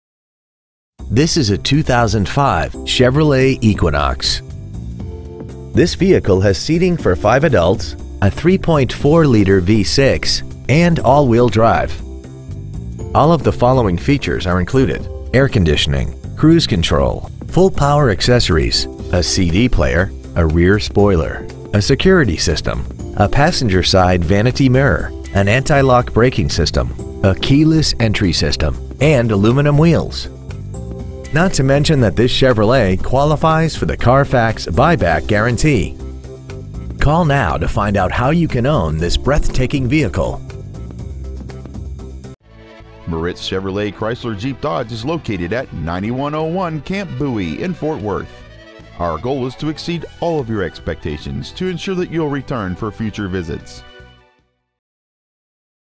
2005 Chevrolet Equinox Dallas TX
Year : 2005
 Make : Chevrolet
 Model : Equinox
 Engine : Gas V6 3.4L/204
 Trans . : Automatic
 Exterior : Black
 Miles : 79,077

 Pre-Owned 2005 Chevrolet Equinox Ft Worth TX